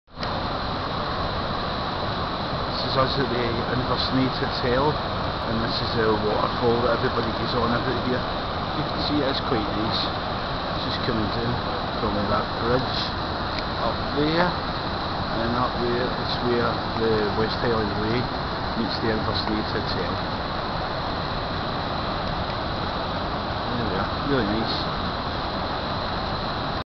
Waterfall at Inversnaid Hotel
A view of the waterfall at the Inversnaid Hotel